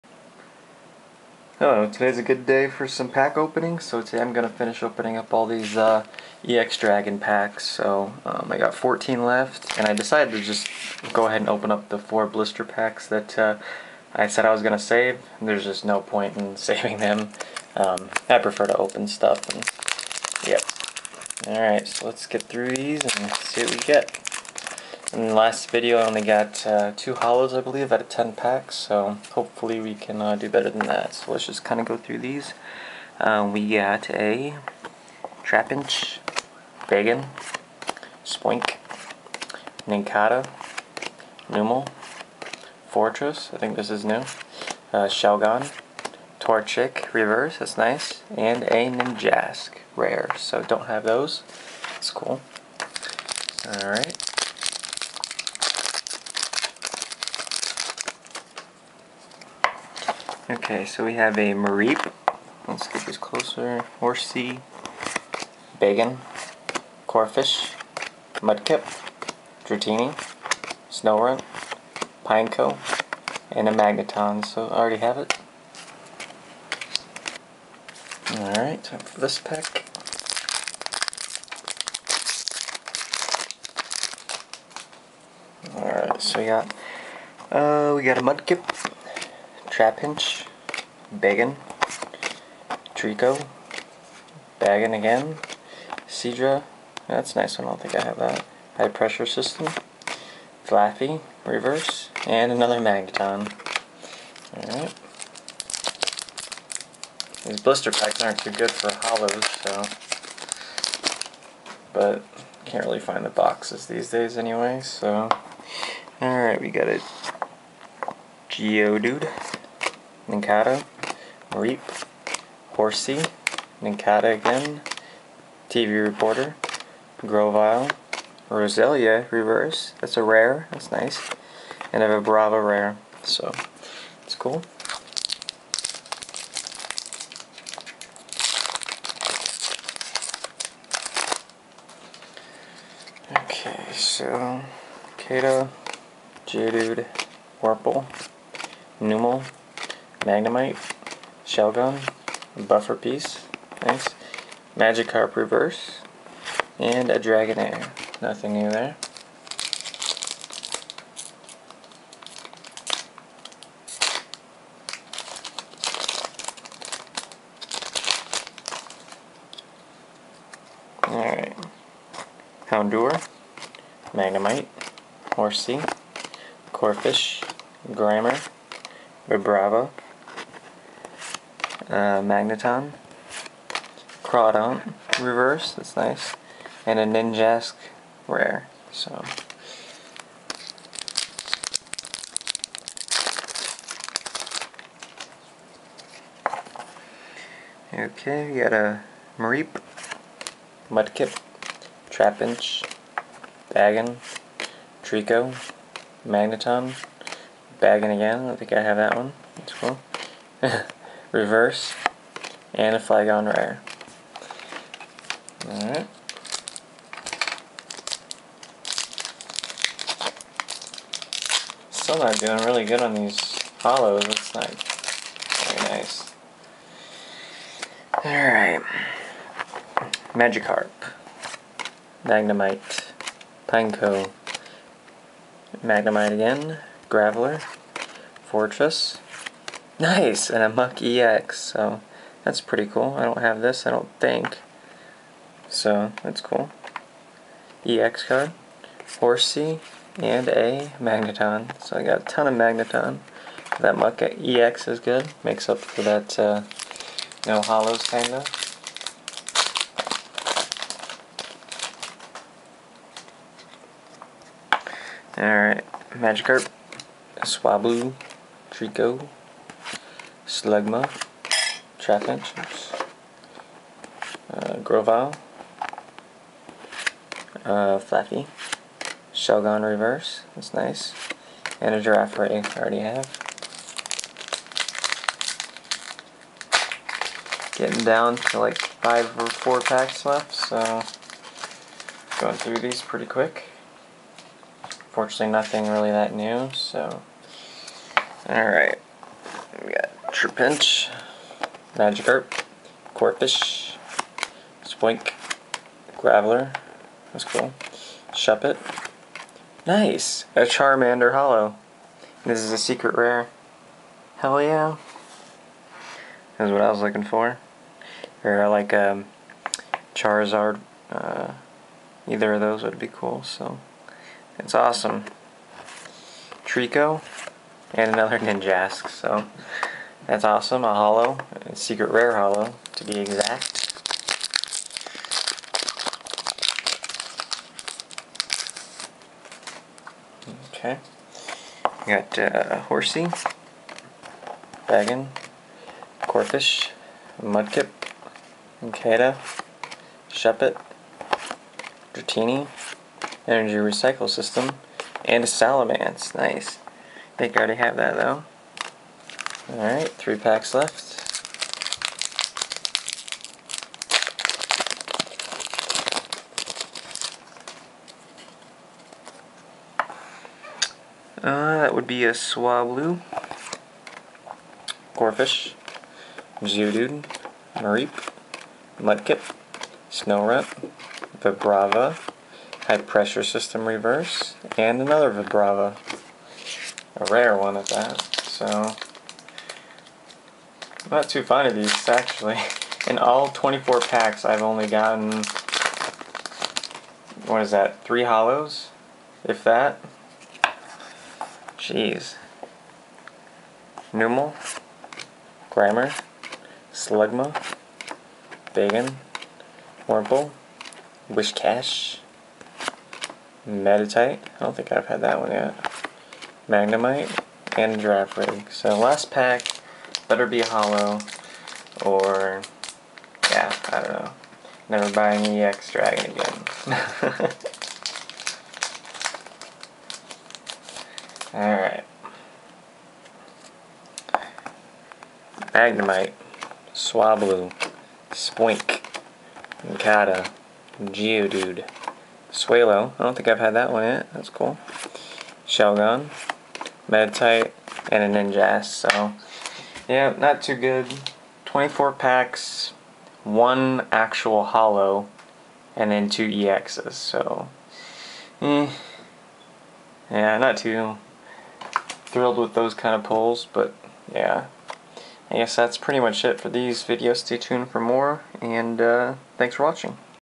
Opening Pokemon EX Dragon Blister Box - Part 2
Second part of the box... pulls just as bad as the first one! Grrrr. Some pretty horrible odds I've ever encountered, 4 holos (including ex's) in 24 packs. Needless to say DO NOT BUY THESE DARN THINGS. Anyhow here it is to finish off the box. :)